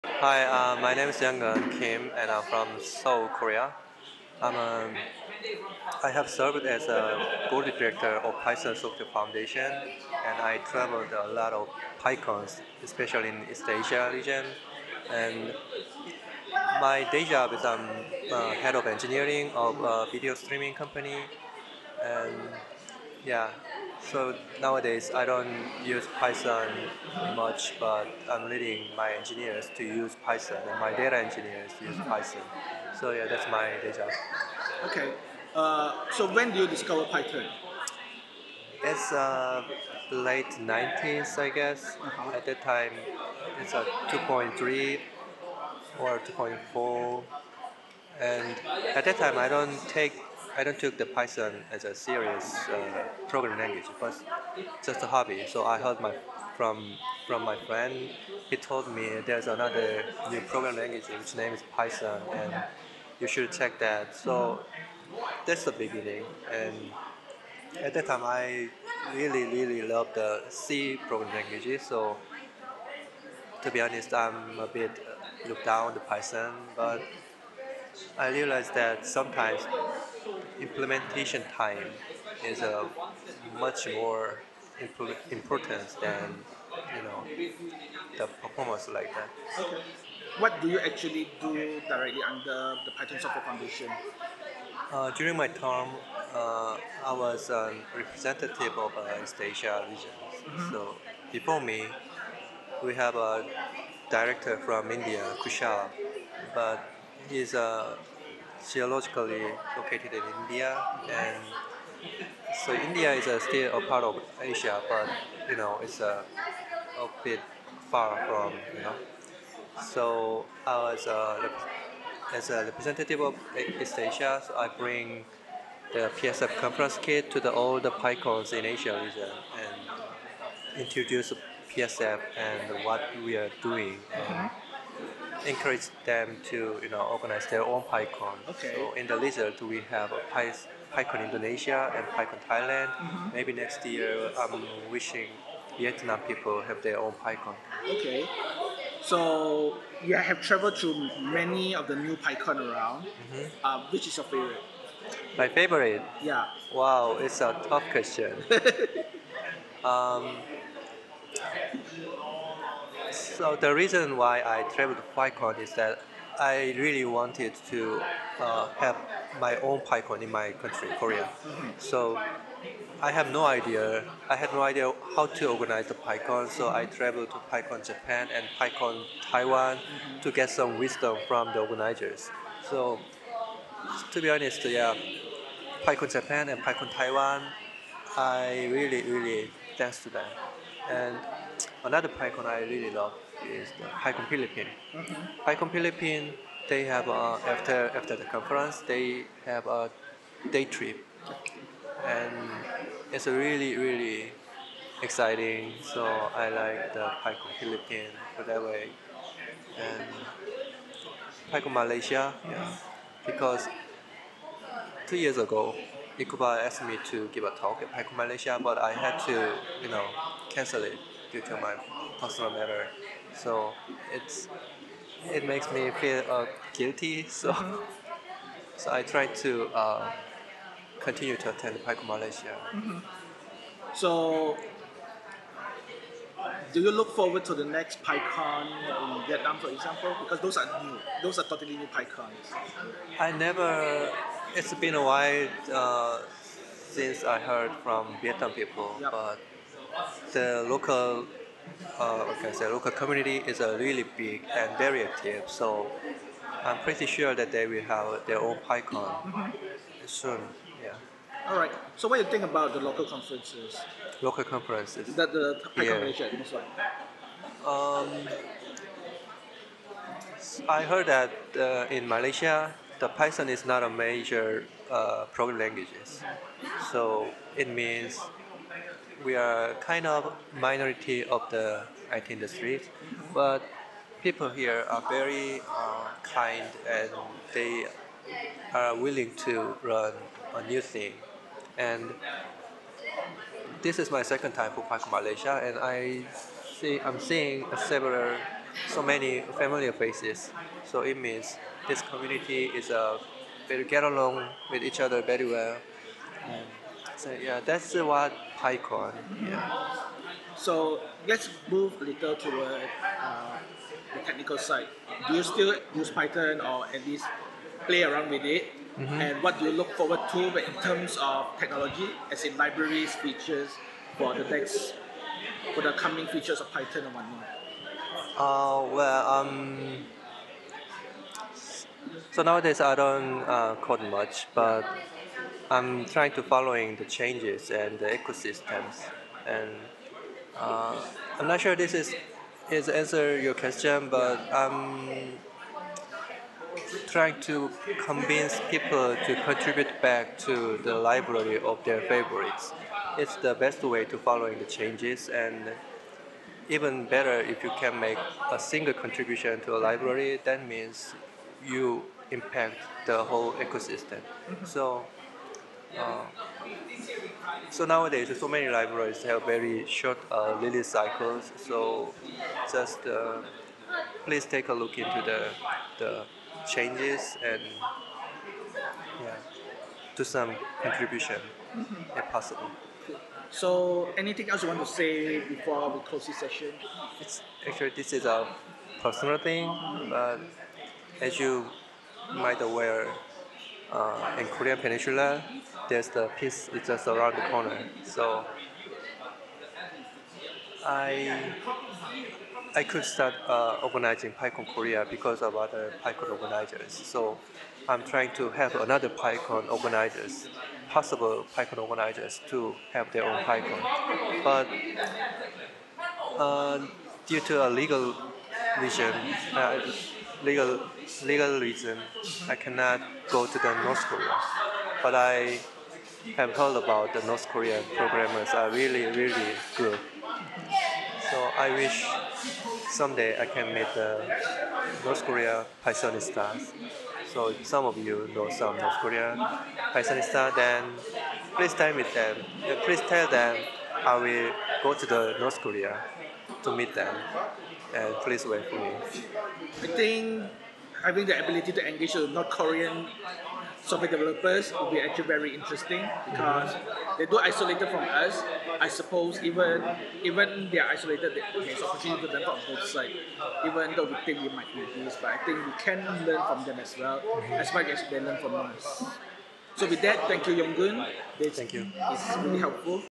About Developer - Kim Younggun
Developer Interview, Kim Younggun.

Interview conducted by Swee Meng during PyCon MY 2018